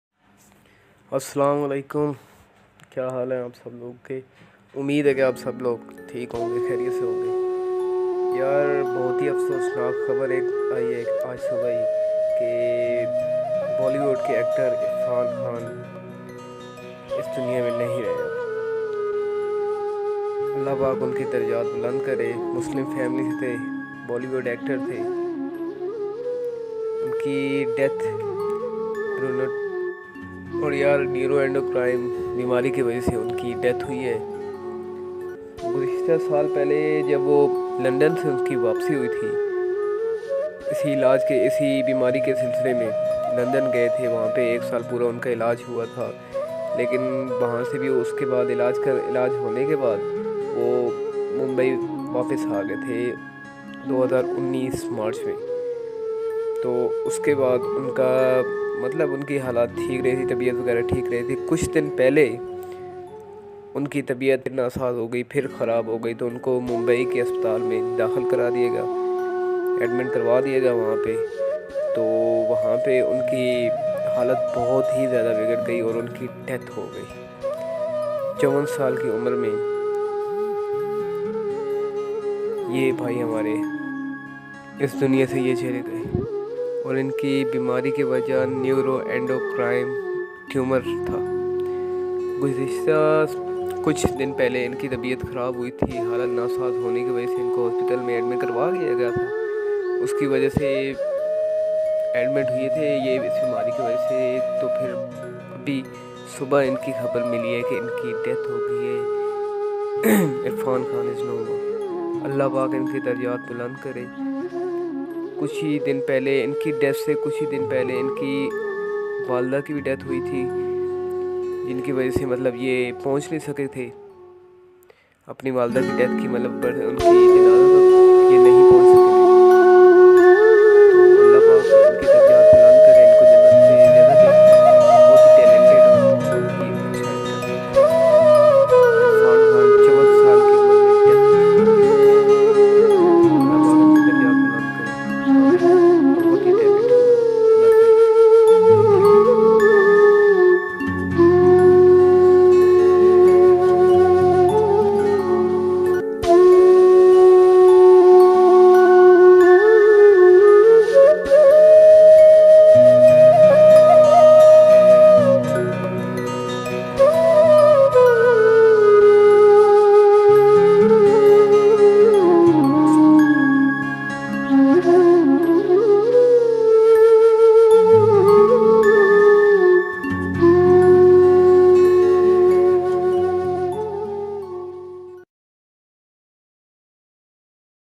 IRFAN KHAN DIED 1967-2020 | CAUSE OF DEATH | REST IN PEACE
Rest in peace Irfan khan the legend May Allah give him a place in jannat ameen